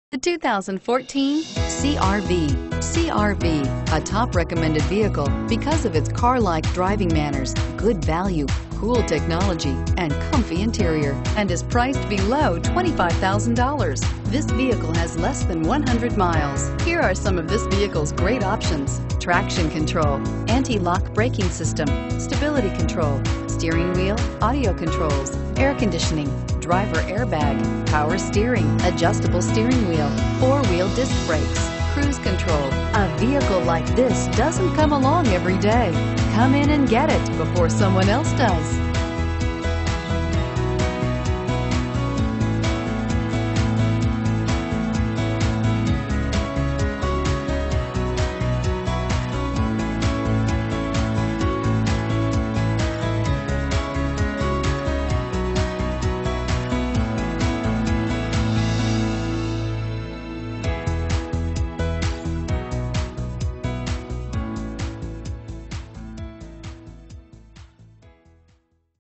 2014 Honda CR-V Buford GA 460638
2014 Honda CR-V LX - $23950

This 2014 Honda CR-V 5dr 2WD 5dr LX SUV features a 2.4L 4 CYLINDER 4cyl Gasoline engine. It is equipped with a 5 Speed Automatic transmission. The vehicle is Polished Metal Metallic with a Gray Cloth interior. It is offered with a full factory warranty.